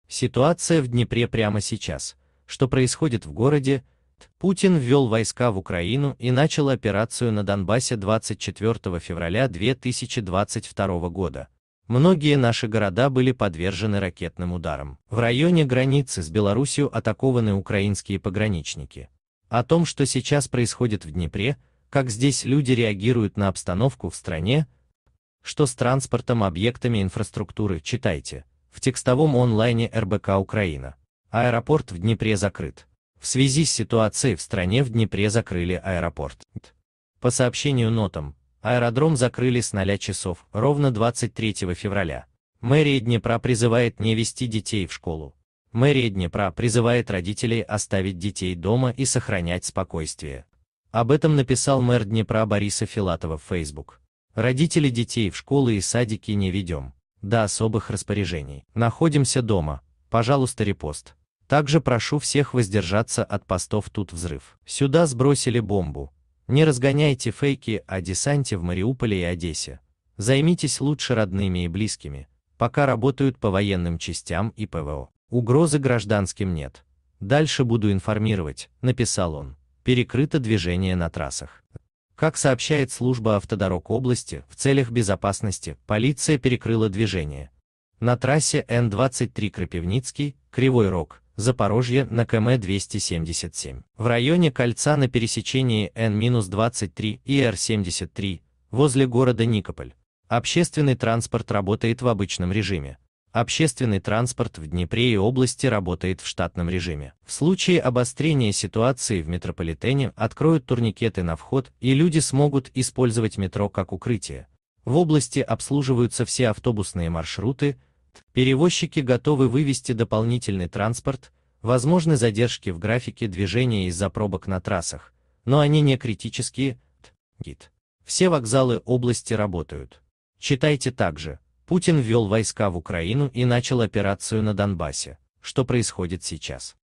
Ситуация в Днепре прямо сейчас: что происходит в городе.
Днепр Путин ввел войска в Украину и начал операцию на Донбассе 24 февраля 2022 года. Многие наши города были подвержены ракетным ударам, в районе границы с Беларусью атакованы украинские пограничники. О том, что сейчас происходит в Днепре, как здесь люди реагируют на обстановку в стране, что с транспортом, объектами инфраструктуры читайте - в текстовом онлайне РБК-Украина. Аэропорт в Днепре закрыт.
В связи с ситуацией в стране в Днепре закрыли аэропорт.  По сообщению NOTAM, аэродром закрыли с 00:00 23 фе